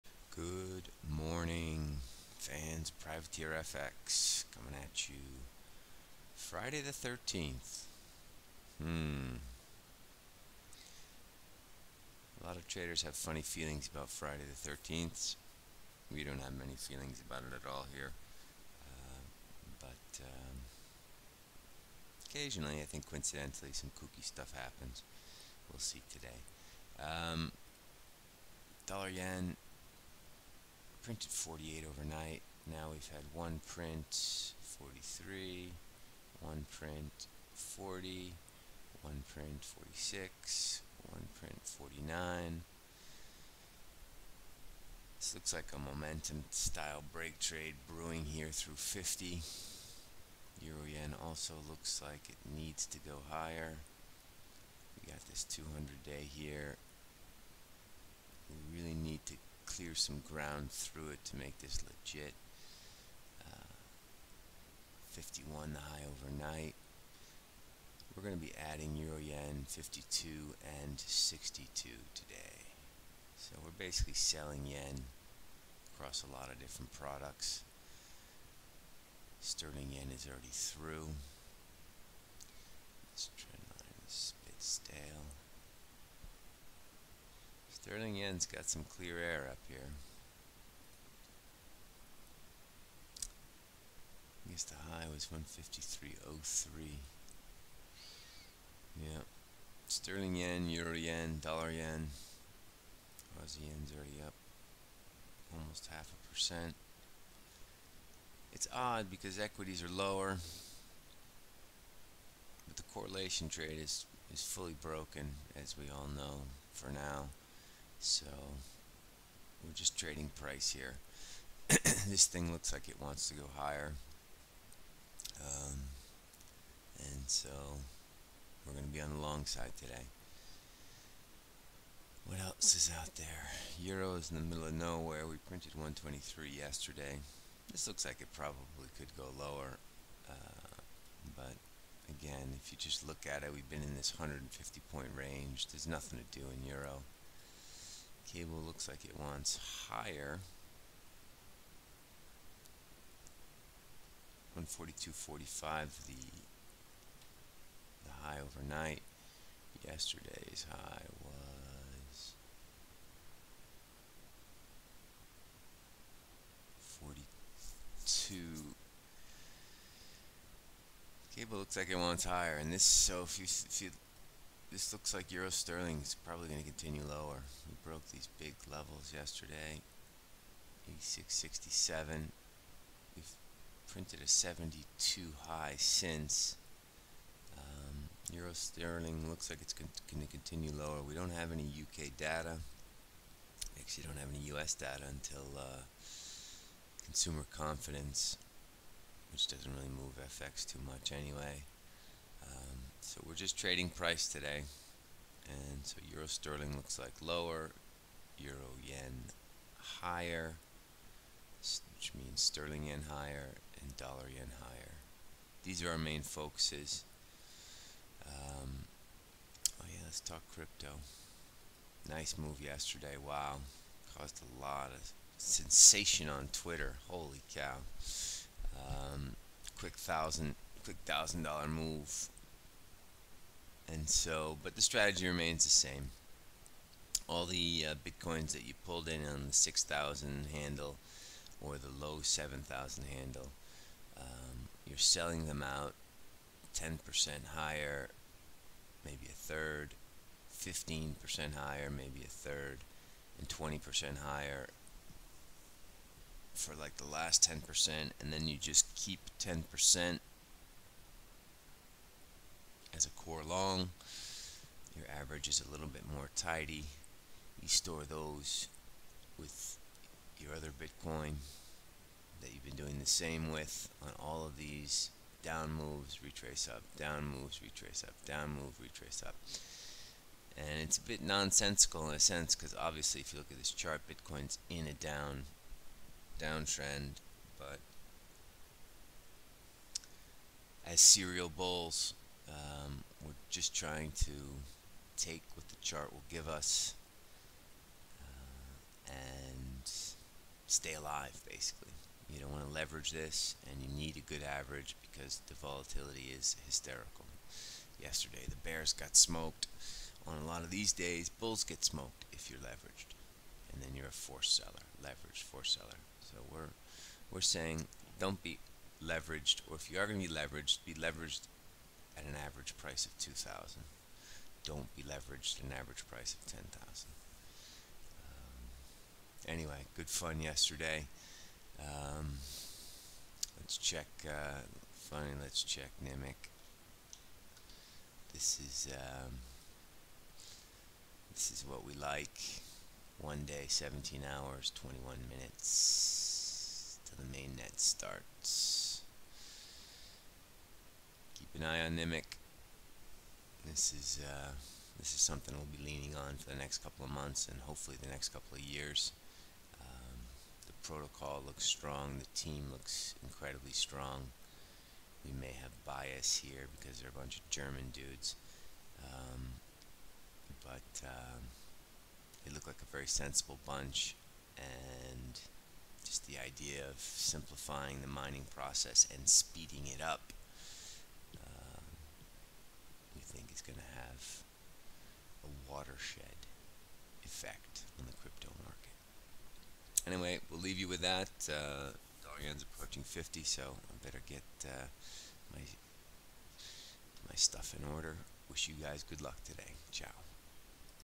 13Apr18 European Opening Call - PrivateerFX
Our view on today's European Open the FX & Crypto markets.

Selling JPY today at open
No news so trading price today in FX

Selling JPY, Break Trade set up
GBPUSD/ Cble looks like it wants higher
EURGBP looks lower

Crypto; BTCUSD same strategy, $Nimiq in 1 day ..

Web: PrivateerFX.com

THIS WAS
13Apr18 European Opening Call - PrivateerFX